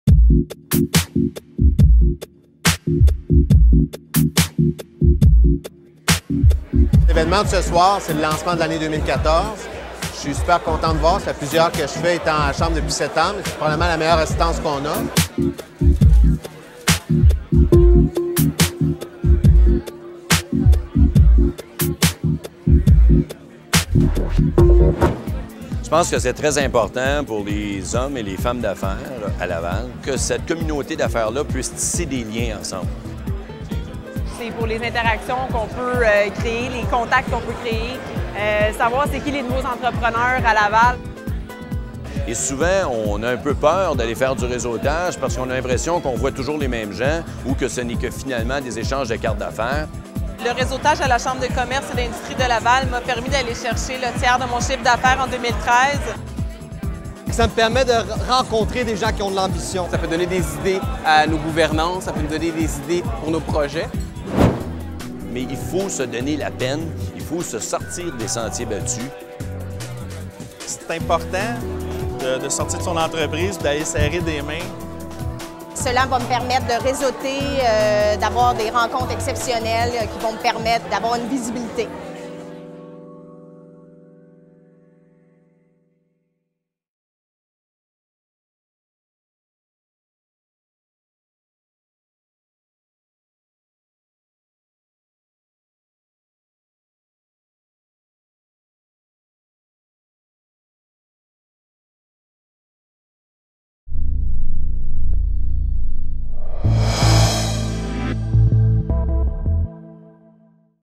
Chambre de commerce et d'industrie de Laval - Événement de la rentrée 2014
Cocktail de la rentrée 2014 de la Chambre de commerce et d'industrie de Laval.